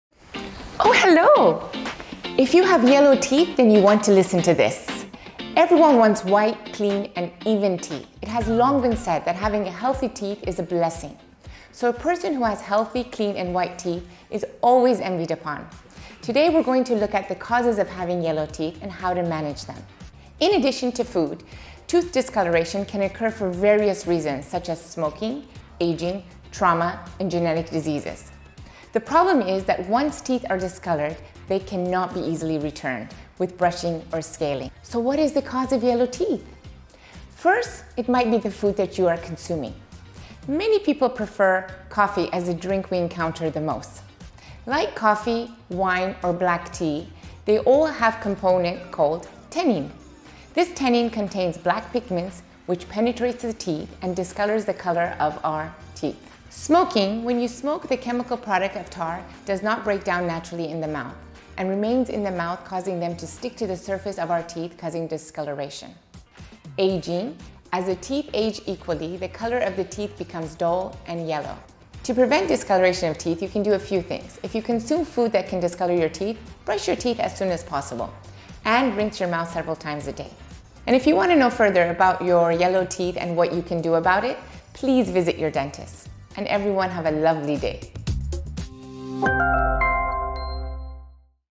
Causes of Yellow Teeth | Dr. Leila Hariri
Four main reasons of Yellow Teeth:
1. Aging
2. Smoking
3. Trauma
4. Genetics Diseases

Address: Villa 410 Al Wasl Road, Dubai